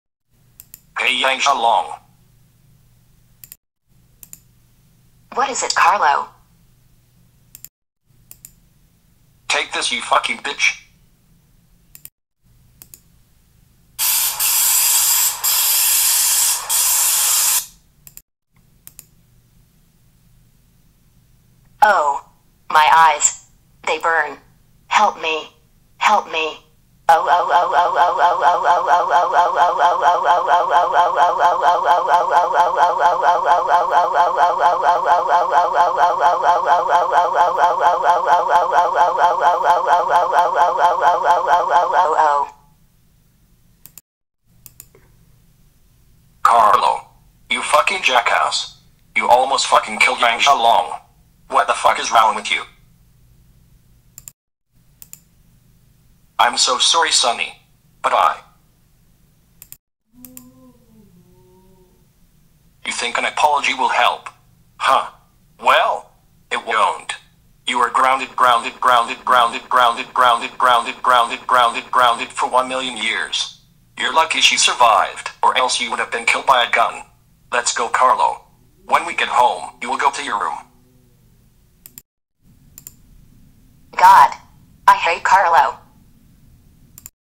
Carlo (The Godfather) pepper sprays Yang Xiao Long and gets grounded
Cast:
Steven as Carlo and Sonny Corleone
Susan as Yang Xiao Long